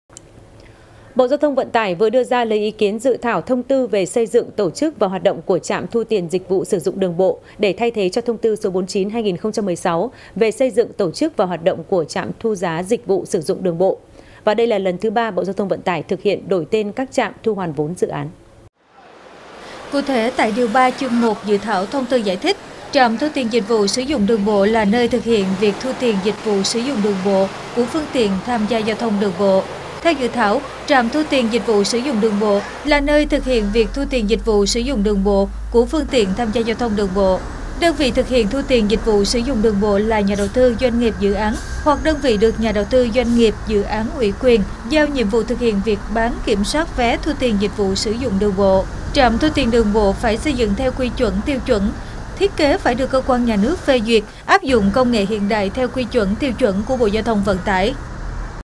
Bộ GTVT đề xuất đổi ‘trạm thu phí” thành “trạm thu tiền’
Bộ GTVT đang lấy ý kiến Dự thảo Thông tư về về xây dựng, tổ chức và hoạt động của trạm thu giá dịch vụ sử dụng đường bộ.